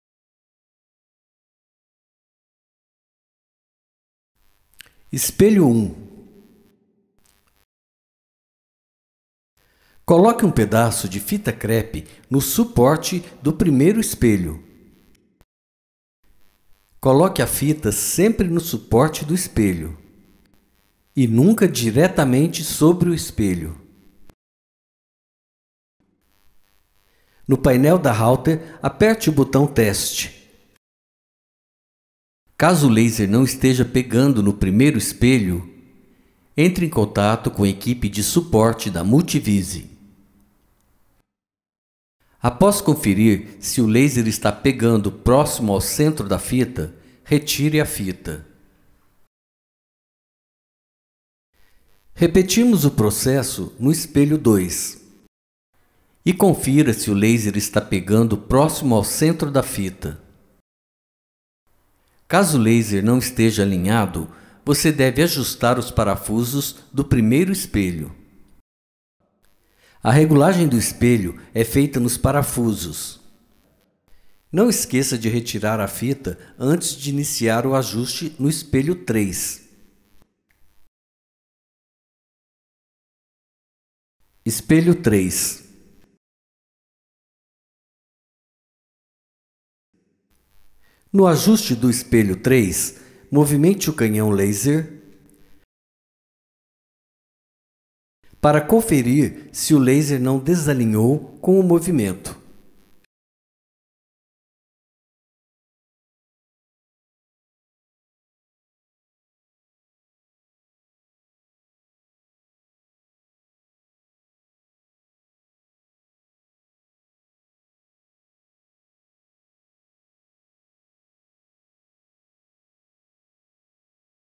Multivisi | Tutorial | Alinhando os espelhos na sua Router Laser VS4040 - 7/7
_________________________________________________________________________________________________________________________________________________________________________________________________________ Nesse vídeo você vai ver como alinhar os espelhos na sua VS4040.

Ficou com dúvidas?